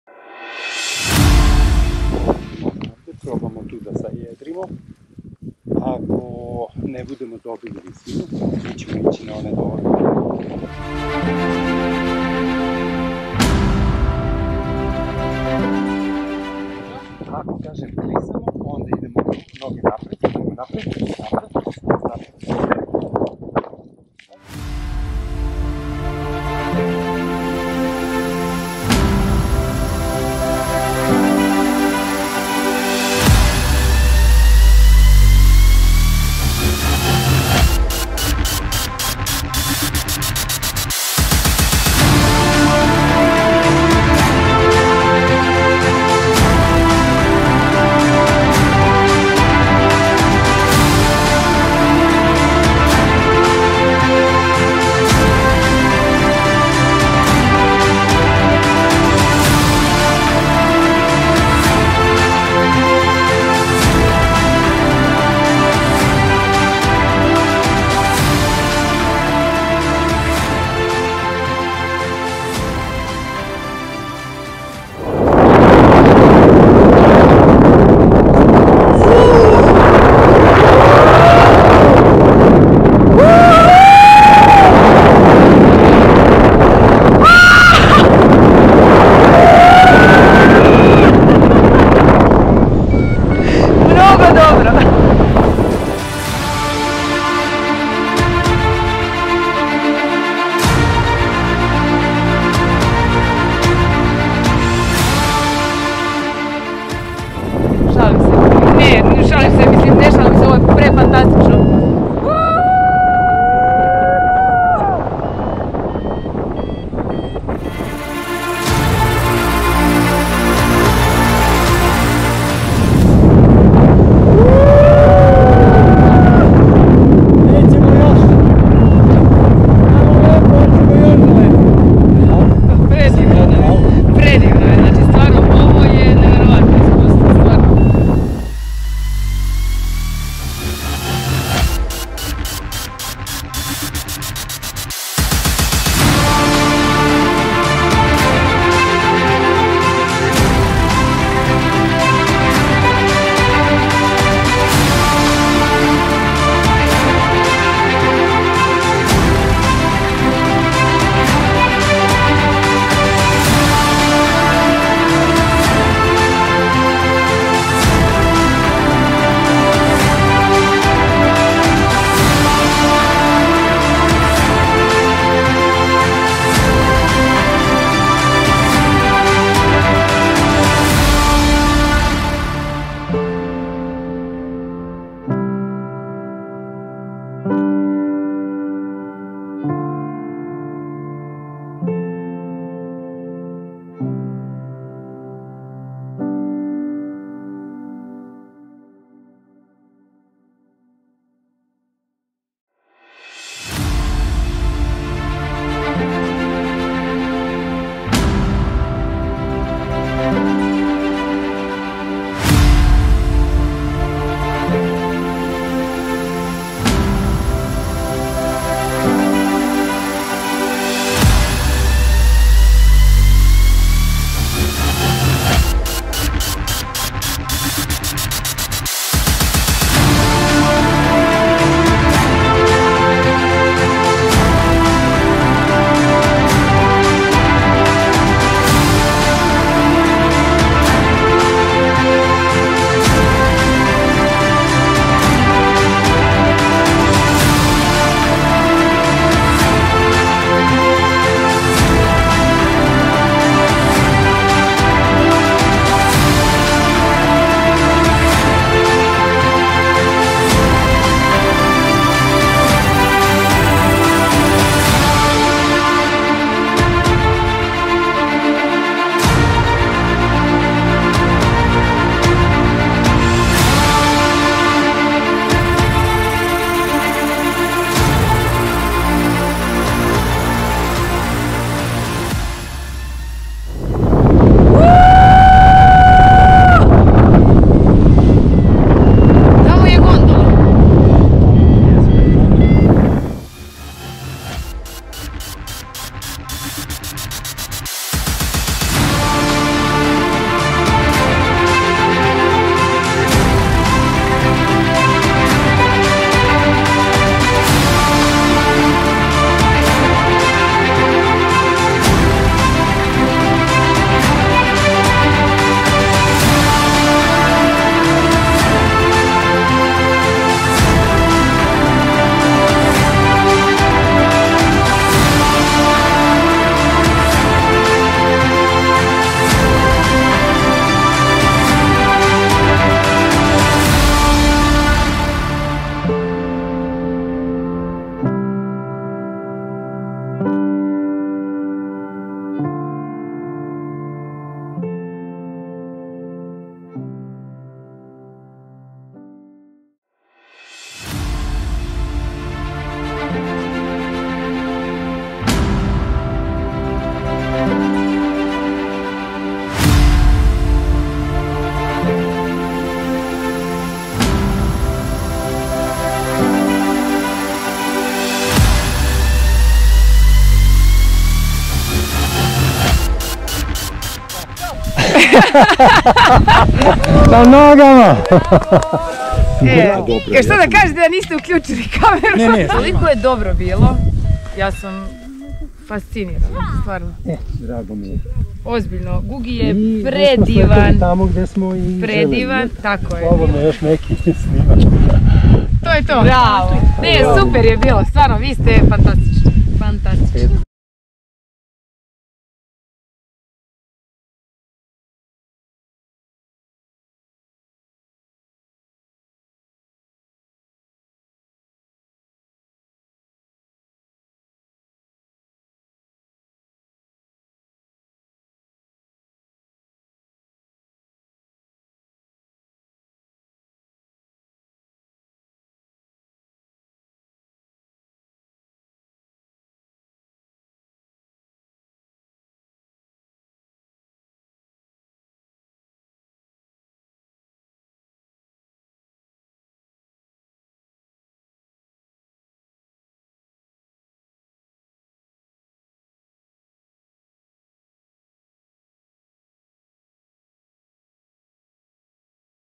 Tandem Zlatibor paraglajding Gold Gondola
Paraglajding Klub ZLATIBOR organizovao je Tandem let na Cigoti tzv. start Vojska Tandem pilot Gugi .